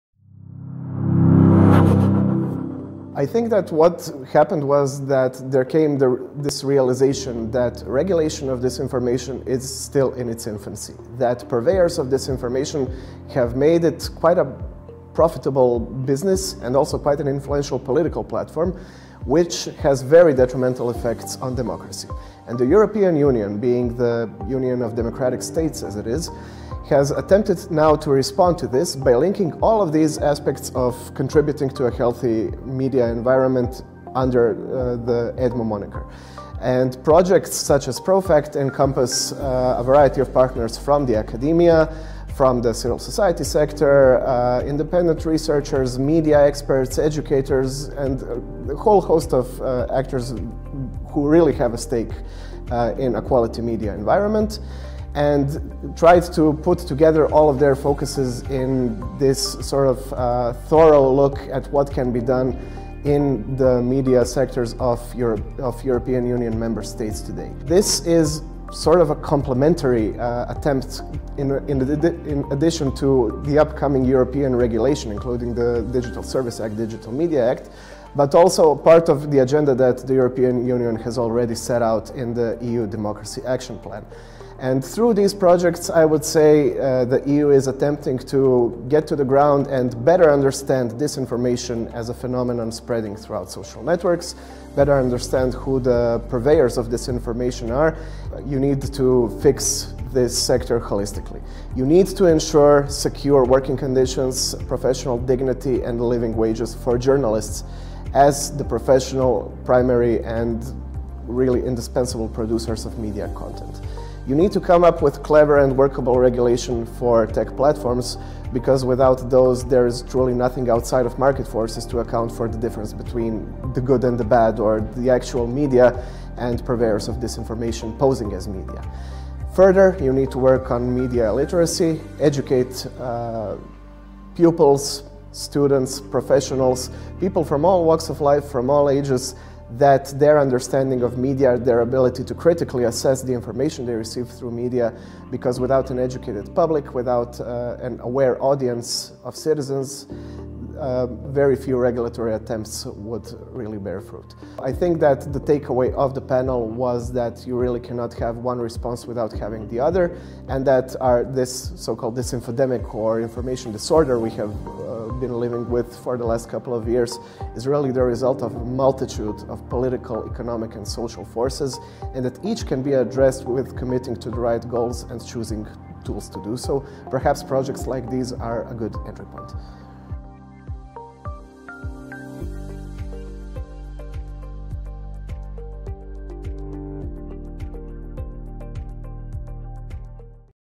Intervju s Draženom Hoffmannom: Do uspješne regulacije dezinformacija putem obrazovane javnosti
U sklopu Gongove konferencije "Europske perspektive: Utjecaj dezinformacija na zdravlje demokracije i digitalno okruženje”, Dražen Hoffmann, Gongov voditelj edukacijskih programa te jedan od sudionika konferencije, je ukazao na važnost stvaranja obrazovane i svjesne javnosti radi osiguranja uspješnosti regulacije dezinformacija na digitalnim platformama.

Konferencija je održana u okviru multidisciplinarnog projekta “Pro-fact: Razotkrivanje COVID-19 dezinformacijskih narativa u Hrvatskoj istraživanjem, provjerom točnosti činjenica i edukacijom”. Projekt istraživanjem, podizanjem svijesti i jačanjem kapaciteta sveobuhvatno pristupa društvenom, političkom i zdravstvenom problemu širenja COVID-19 dezinformacijskih kampanja, a provode ga zajedno Gong, Fakultet političkih znanosti u Zagrebu, Sveučilište u Dubrovniku, Fakultet elektrotehnike i računalstva, Faktograf sa suradnicama Hrvatskim novinarskim društvom (HND) i Sindikatom novinarki Hrvatske (SNH).